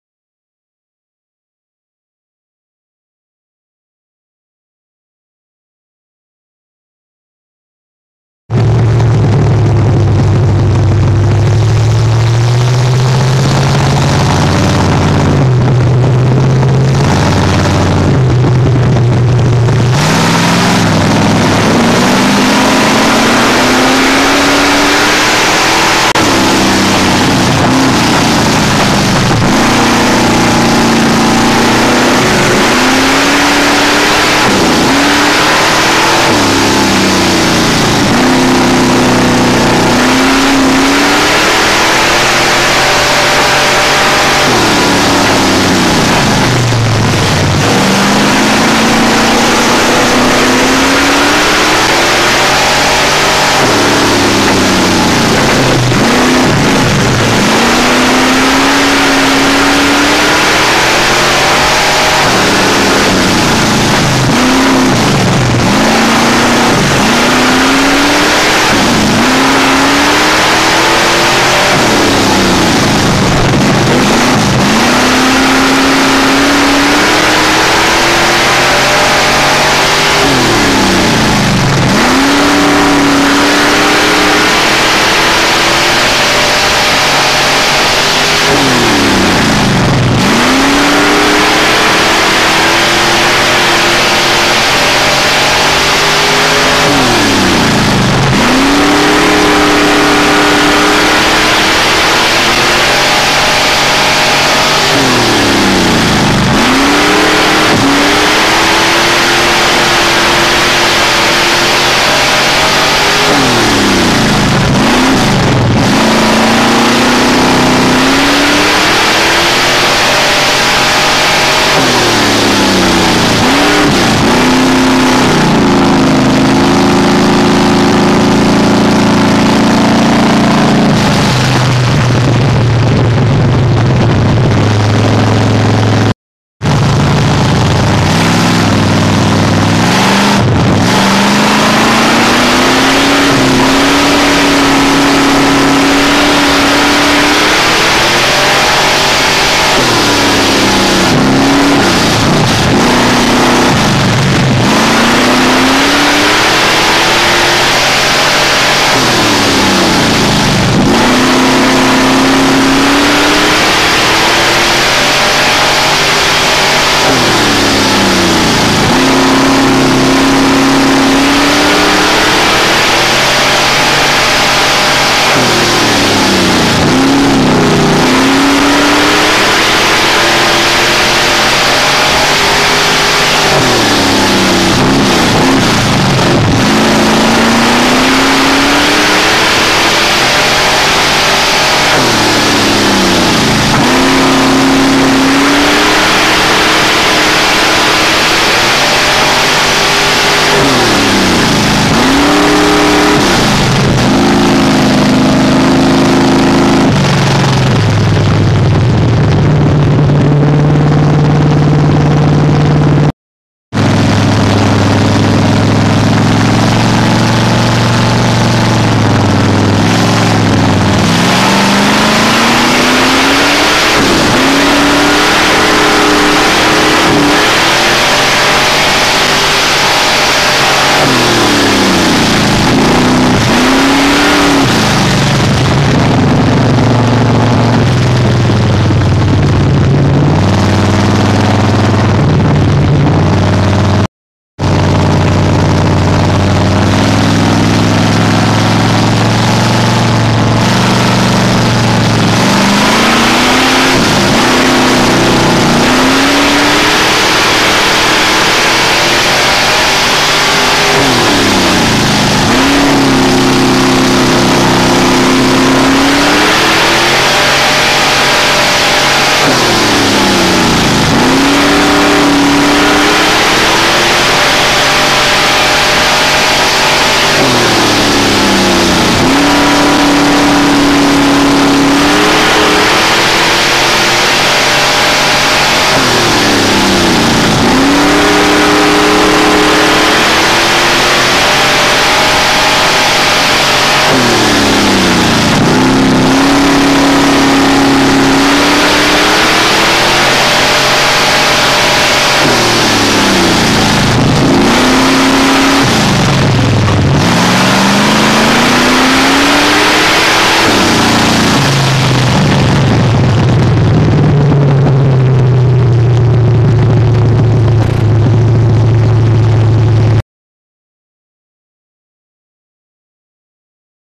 7 21 07 B feature street stock adrian speedway missouri
Drew pole, had flat after 1 lap in  heat, to the back of the B I go.